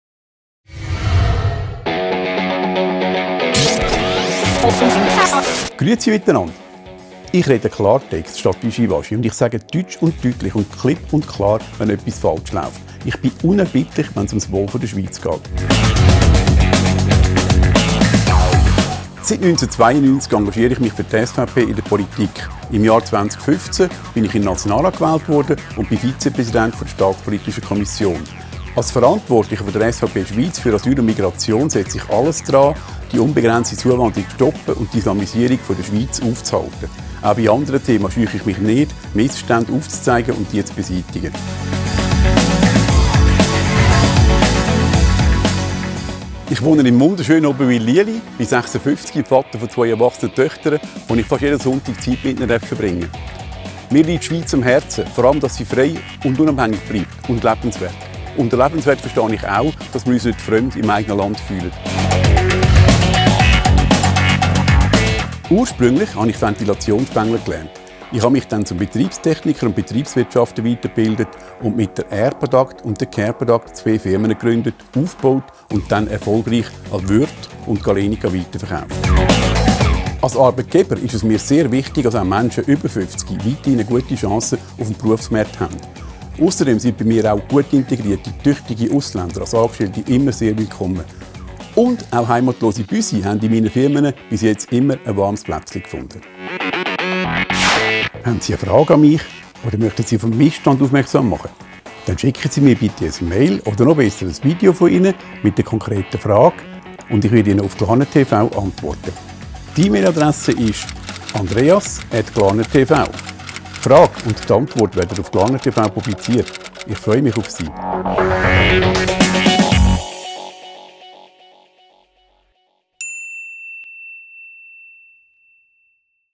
Glarner.tv: Andreas Glarner - Mensch - Unternehmer - Politiker
Dieses Video zeigt Andreas Glarner in seinen verschiedenen Rollen und zeigt ein anderes als das von den Medien oft zelebrierte Bild.
Andreas Glarner spricht Klartext statt wischiwaschi und sagt klipp und klar, wenn etwas falsch läuft.

«Grüezi mitenand
Ich spreche Klartext statt Wischiwaschi und ich sage deutsch und deutlich und klipp und klar wenn etwas falsch läuft - ich bin unerbittlich wenn es um das Wohl der Schweiz geht.»
 «Seit 1992 engagiere ich mich für die SVP in der Politik. Im Jahr 2015 wurde ich in den Nationalrat gewählt und bin Vizepräsident  der staatspolitischen Kommission. Als Verantwortlicher der SVP Schweiz für Asyl und Migration setze ich alles daran, die unbegrenzte Zuwanderung zu stoppen und die Islamisierung der Schweiz aufzuhalten. Auch bei anderen Themen scheue ich mich nicht, Missstände aufzuzeigen und diese zu beseitigen»
Ich wohne im wunderschönen Oberwil-Lieli, bin 56 Jahre alt und Vater von zwei erwachsenen Töchtern, mit denen ich fast jeden Sonntag Zeit verbringen darf.
Mir liegt die Schweiz am Herzen, vorallem dass sie frei und unabhängig bleibt, und lebenswert. Unter lebenswert verstehe ich auch, dass wir uns nicht fremd im eigenen Land fühlen.
«Ursprünglich habe ich Ventilationsspengler gelernt. Ich habe mich zum Betriebstechniker und Betriebswirtschafter weitergebildet und mit der Airproduct und Careproduct zwei Firmen gegründet, aufgebaut und diese dann erfolgreich an Würth und Galenica weiterverkauft. Als Arbeitgeber ist es mir sehr wichtig, dass auch Menschen über 50 weiterhin eine gute Chance auf dem Berufsmarkt haben. Ausserdem sind bei mir auch gut integrierte, tüchtige Ausländer als Angestellte immer sehr willkommen. Und - auch heimatlose Büsi haben in meinen Firmen bisher immer einen warmen Platz gefunden.